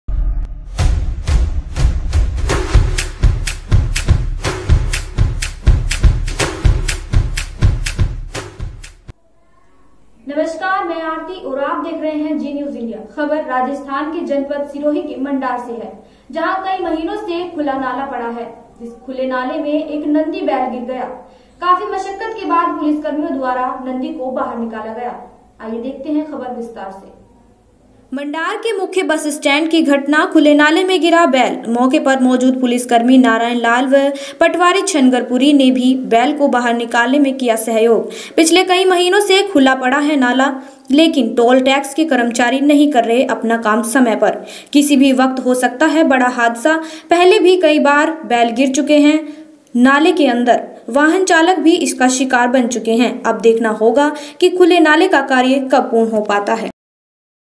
खुले नाले में गिरा नंदी बैल , काफी मशक्कत के बाद निकाला बाहर
मंडार के मुख्य बस स्टैंड की घटना । खुले नाले में गिरा बेल मौके पर मौजूद पुलिसकर्मी नारायण लाल व पटवारी छगन पुरी ने भी बेल को बाहर निकालने में किया सहयोग । पिछले कई महीनों से खुला पड़ा है नाला लेकिन टोल टैक्स के कर्मचारी नहीं कर रहे हैं अपना कार्य समय पर । किसी भी वक्त हो सकता है बड़ा हादसा।  पहले भी कई बार बेलगिरी चुके हैं ना लेके अंदर वह वाहन चालक भी इसका शिकार बन चुके हैं । अब देखना होगा कि खुले नाले का कार्य कब पूर्ण हो पाता है ।